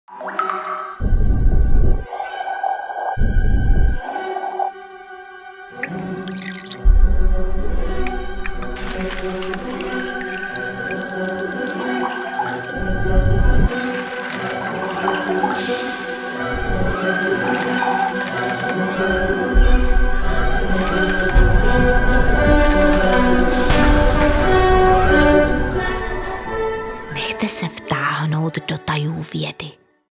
Přijďte na Veletrh vědy 2017! (1)
Výlet do vesmíru, exkurze do jaderného reaktoru díky virtuální realitě, největší dron ve střední Evropě v akci, Klamárium plné optických klamů či návštěva středověkého Pražského hradu prostřednictvím detailních animací – to jsou jen střípky z programu Veletrhu vědy. Veletrh nabídne také kariérní zónu s informacemi o pracovním uplatnění absolventů, zónu pro pedagogy, veselou vědu pro nejmenší, setkání s vědci, workshopy či přednášky. Letošní novinkou budou také panelové diskuse na témata současné společnosti a špičkového výzkumu s moderátorem Václavem Moravcem a předními odborníky české vědy. Veletrh vědy se uskuteční od 8. – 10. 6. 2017 V PVA v Letňanech. Vstup i parkování ZDARMA.